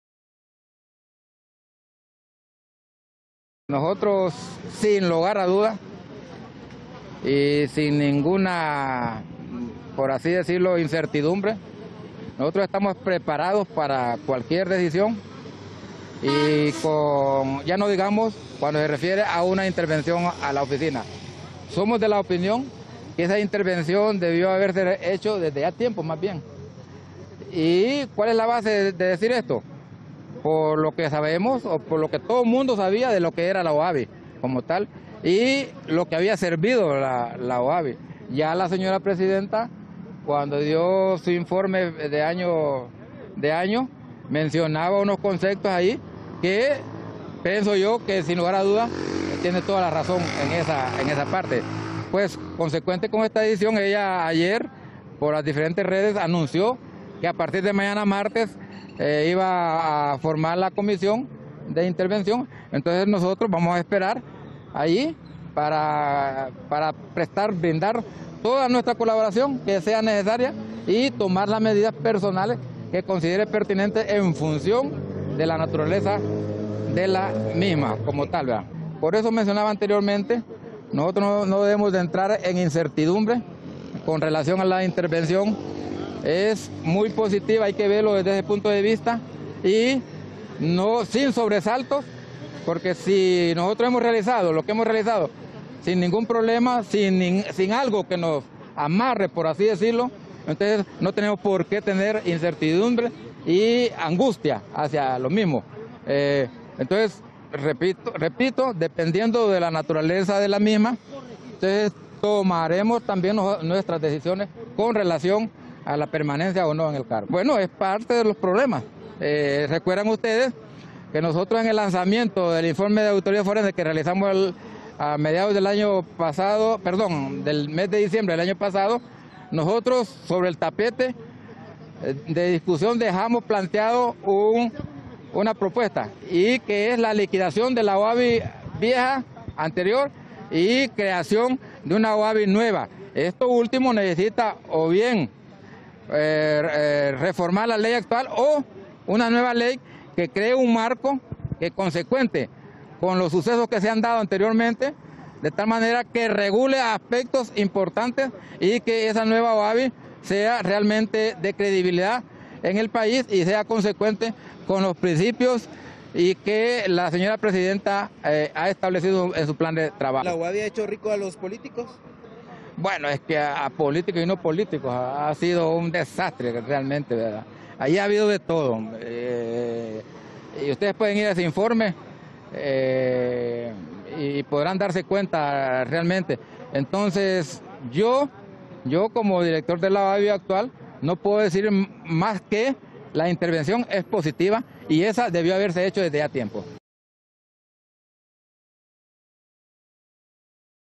Director de la OABI “intervención se debió haber dado desde administraciones pasadas”
La presidenta de Honduras, Xiomara Castro, anunció este  que el próximo martes 7 de febrero ordenará la integración de dos comisiones interventoras, una en la Oficina Administradora de Bienes Incautados  y otra en la Dirección General de Servicio Civil. Entre tanto, el director de la OABI afirmó que la acción se debió haber hecho desde administraciones pasadas.